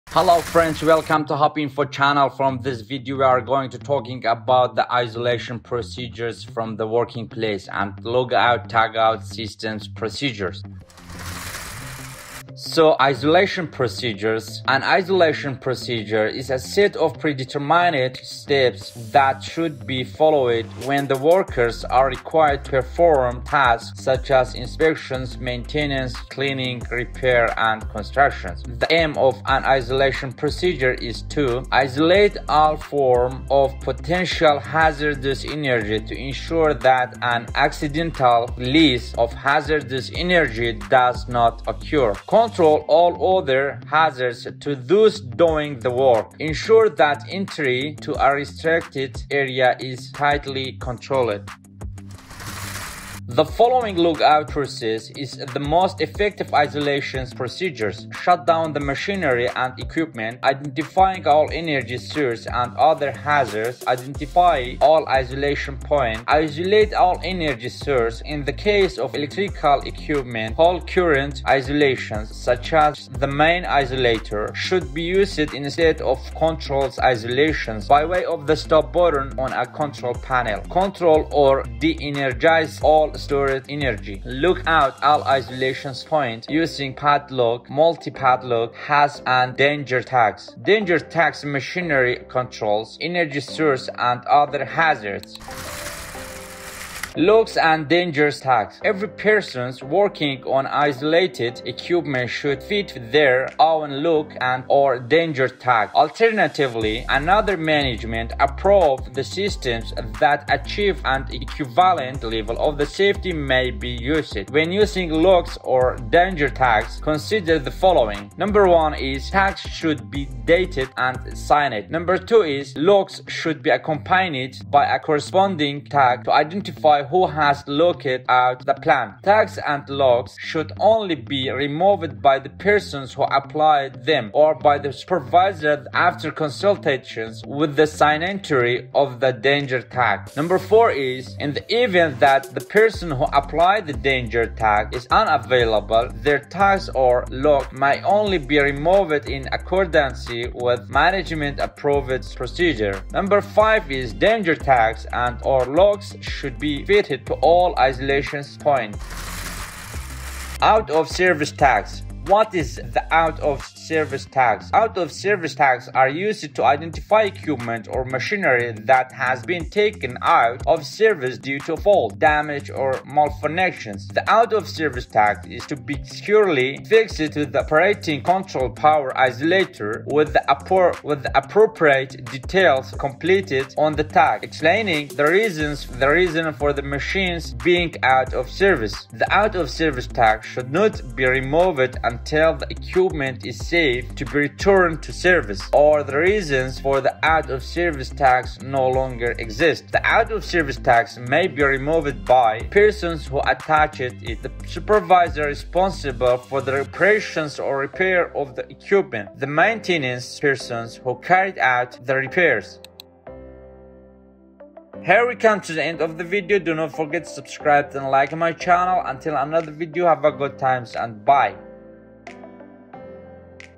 Lock Out Tag Out Safety System | LOTO Procedures | Isolation Procedure
All about Lock Out Tag Out Safety System | LOTO Procedures | Isolation Procedure| LOTO Safety at working place in English language  with simple explanation Example.

.to get more knowledge.

Thanks & Regards,
HOPE INFO